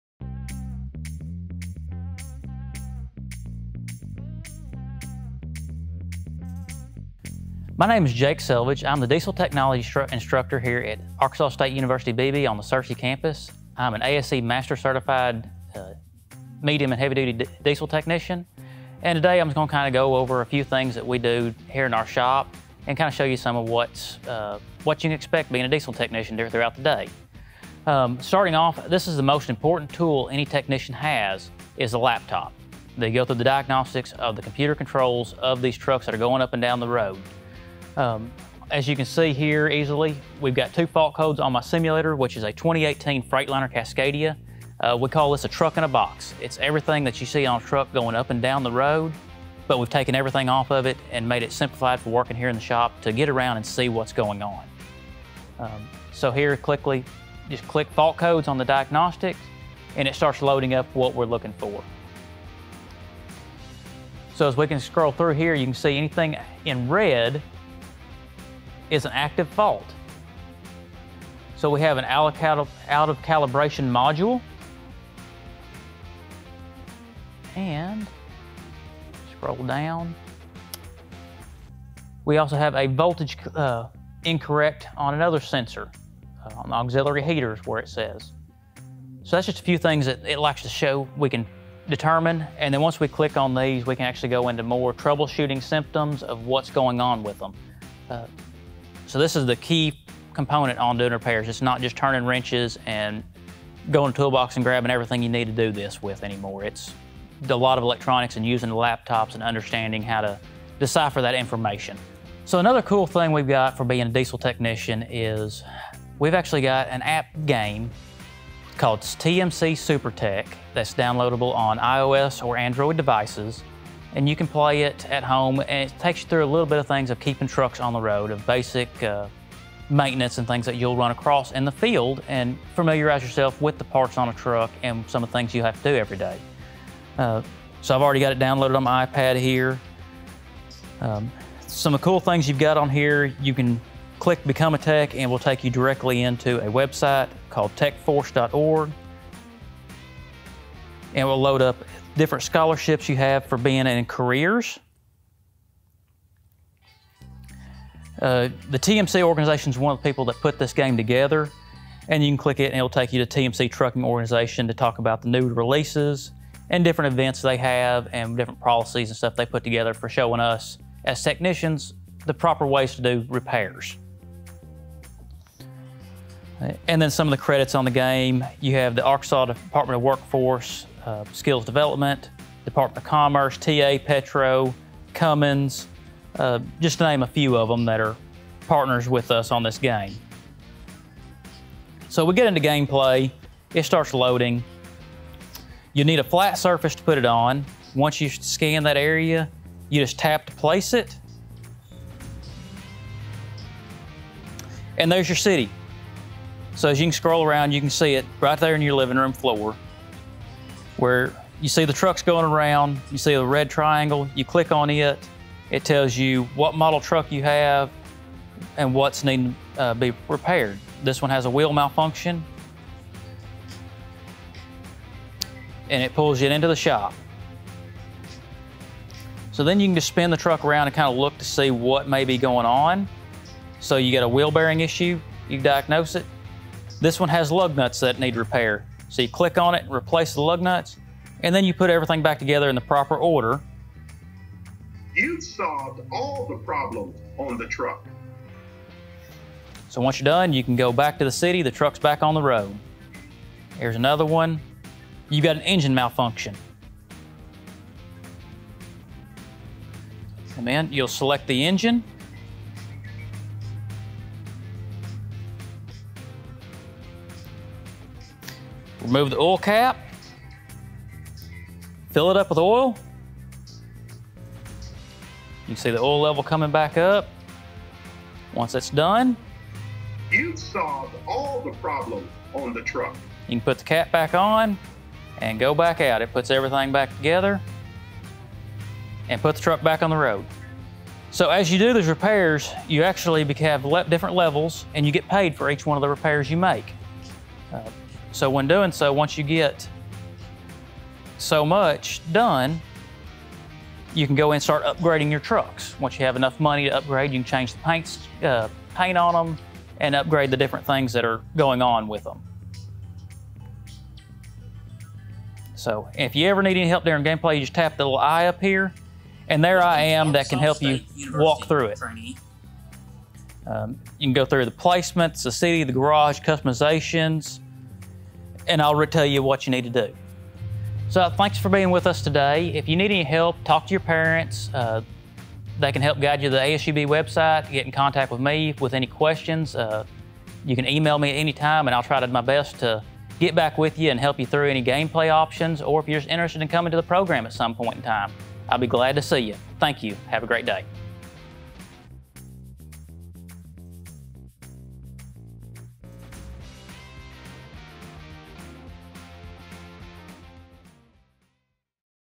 Vanguard Discovery Camp - A Day in the Life of a Diesel Technician
Want to learn more about what a Diesel Technician does? Join our very own Jake Selvidge as he walks you through the process using a video game. In this session, Jake will show you how to use the app and simulators in his shop, demonstrating a day in the life of a Diesel Technician.

✅ Apply today!